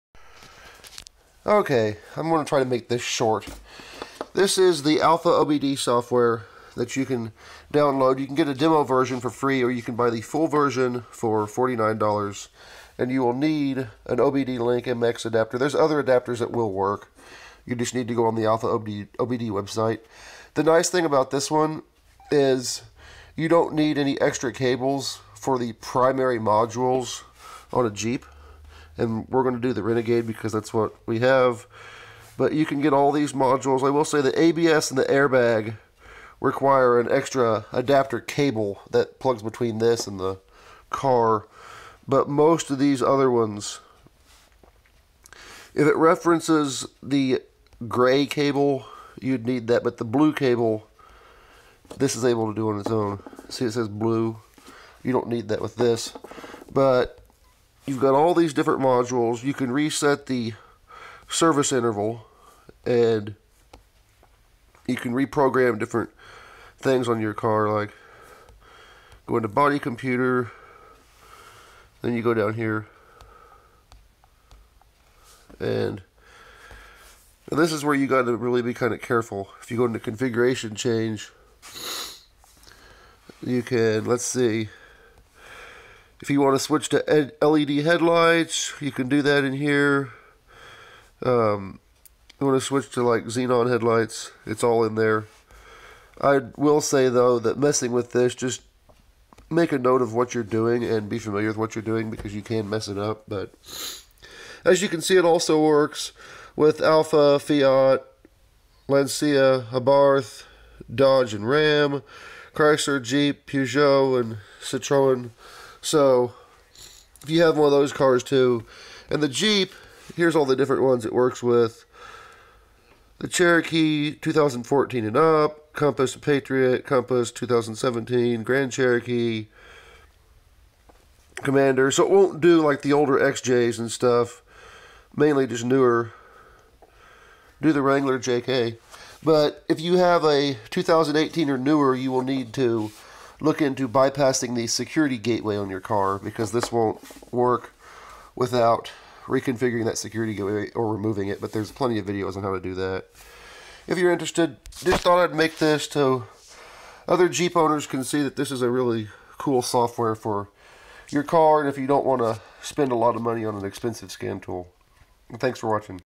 Alfaobd for Jeep Renegade and other FCA cars
This is a brief demonstration of alfa OBD and how you can use it to diagnose your Fiat Chrysler vehicle